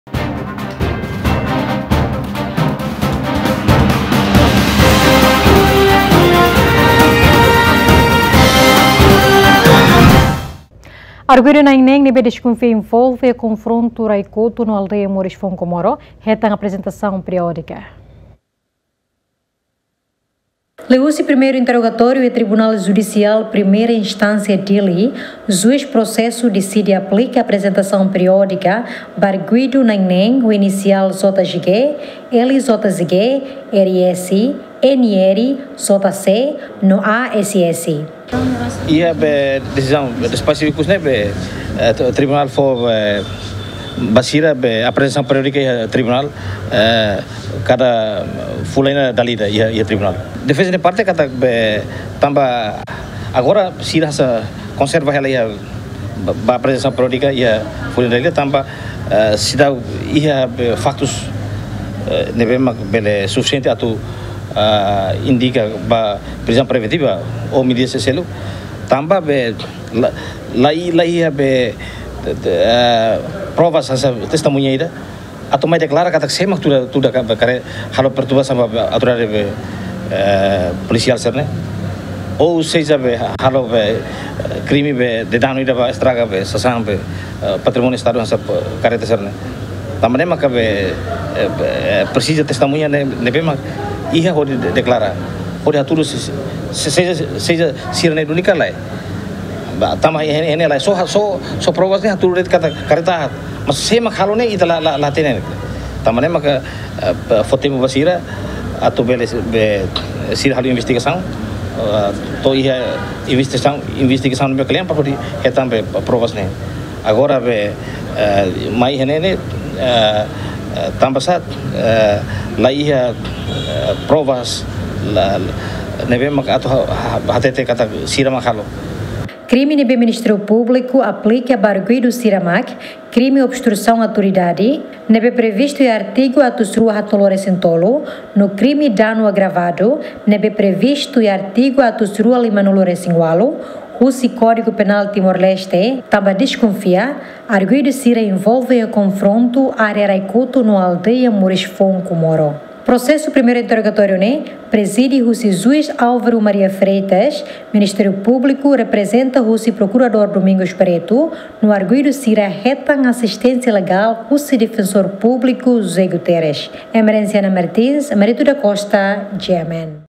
Konfrontu iha Rai Kotu, Arguido nain 6 hetan Aprezentasaun Periódika
Arguido nain 6 ne’ebé envolve iha konfrontu Rai Kotu hetan Aprezentasaun Periódika